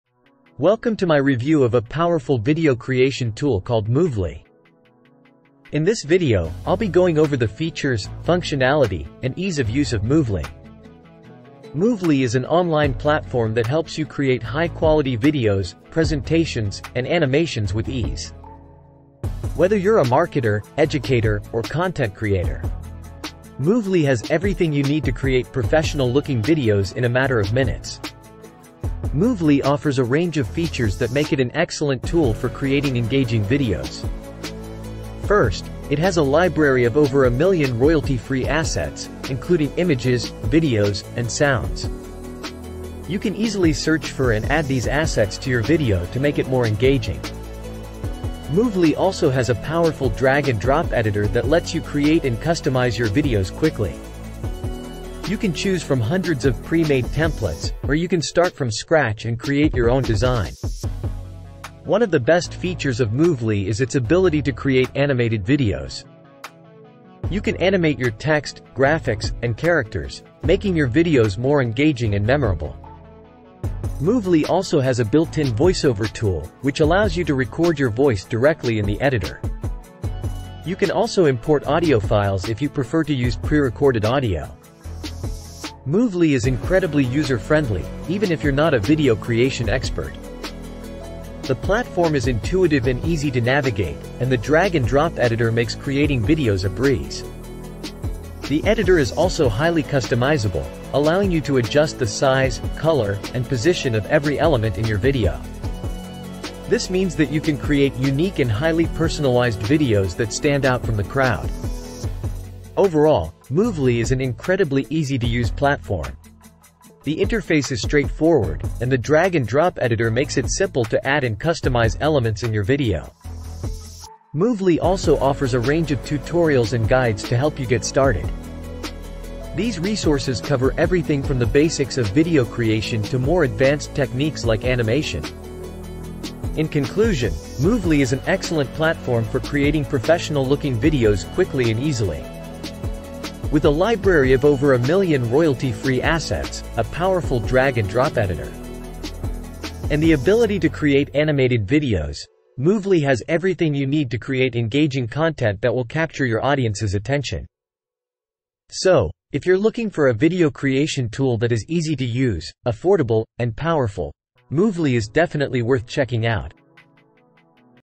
Unleash your creativity with Moovly! | In-depth review
Moovly is a cloud-based video creation platform that allows users to create animated videos, explainer videos, presentations, and other types of multimedia content easily. In this review, we'll take a closer look at Moovly's features, pricing, and ease of use to help you decide if it's the right video creation tool for your needs.